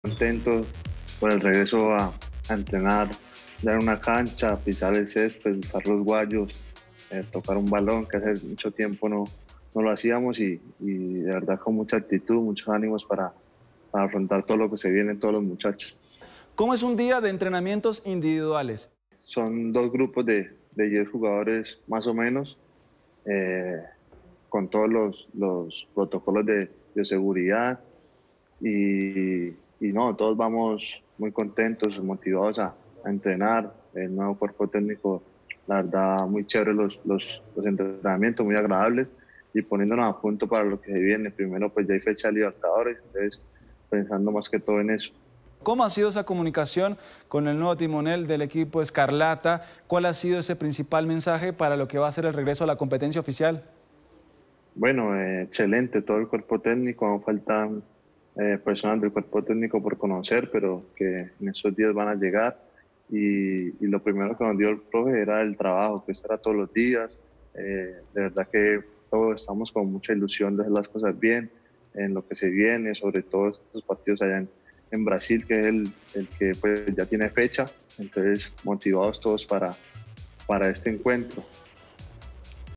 Liga BetPlay | Estoy contento con el regreso a entrenamientos: Cristian Arrieta
El defensa del conjunto ‘escarlata’ también se refirió al nuevo cuerpo técnico y afirmó que tienen mucha ilusión.